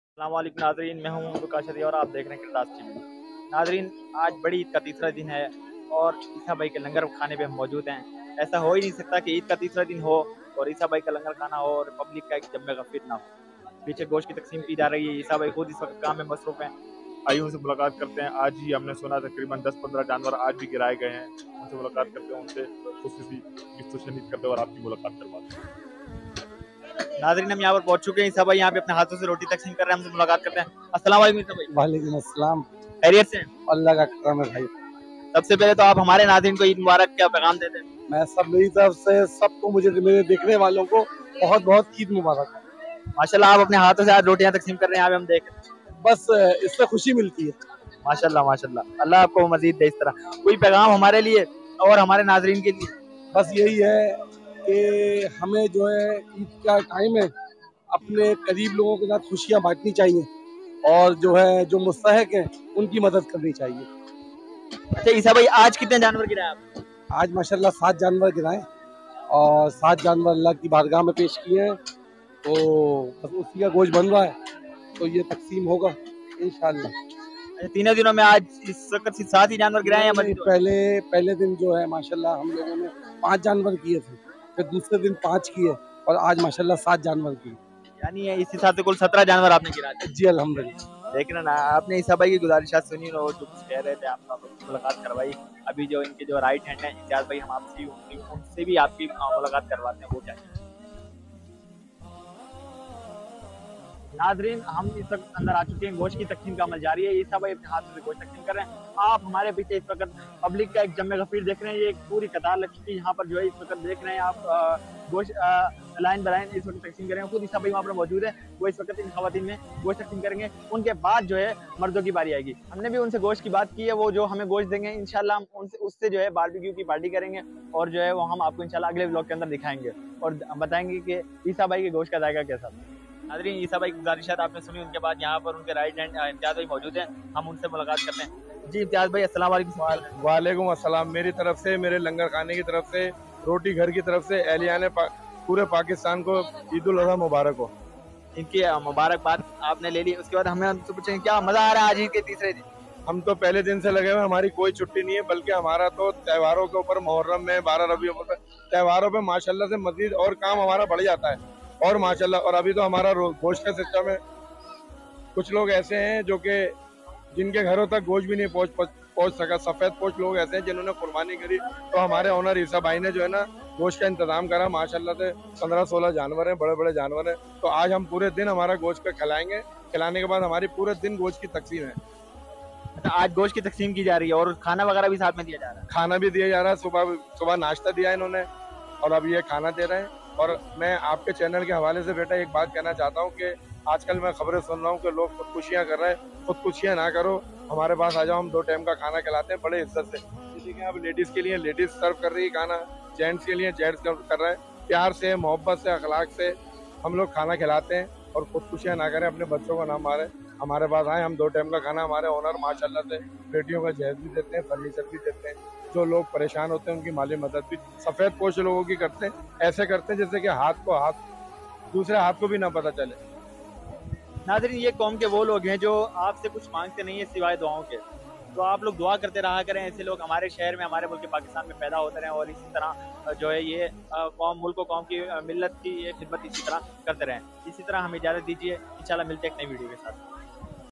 Eissa Bhai ney 15 janwaro ki qurbani ki | gareebo my gosht taqseem kiya | Qirtaas Tv
qurbani ka goshtqurbani ke masailqurbani ka gosht ki taqseemQurbani ka gosht taqseem karne ka tarikabakra mandi